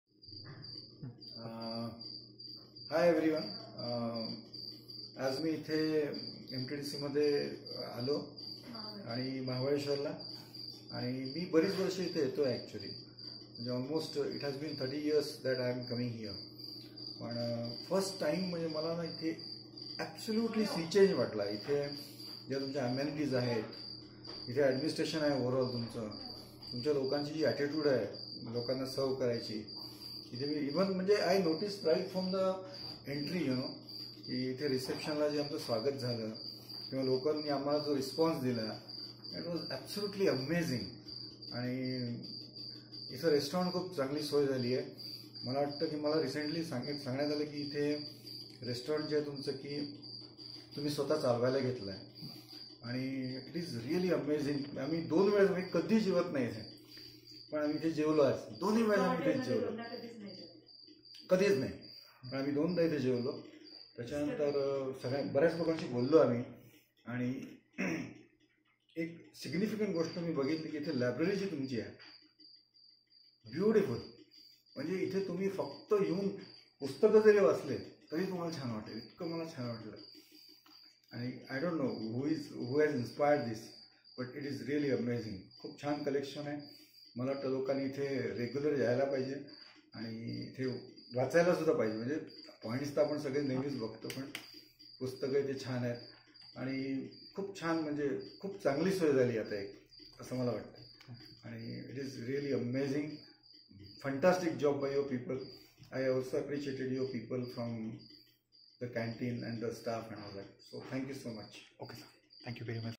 Guest Feedback
Mr Mundle, our esteemed Guest Shares his 30 years experience of his stays at MTDC Resort Mahabaleshwar, where he spoke about positive changes and improved services and facilities at MTDC Resort Mahabaleshwar

माननीय पर्यटक श्री मुंडले यांनी महाबळेश्वर पर्यटक निवास व रेस्टॉरंट बद्दल आपल्या ३० वर्षाचे अनुभव व्यक्त केले आहे. ज्यामध्ये त्यांनी MTDC महाबळेश्वर रिसॉर्ट मद्यें झालेल्या आमूलाग्र बदलांचे व सोयीसुविधांची वर्णन केले आहे.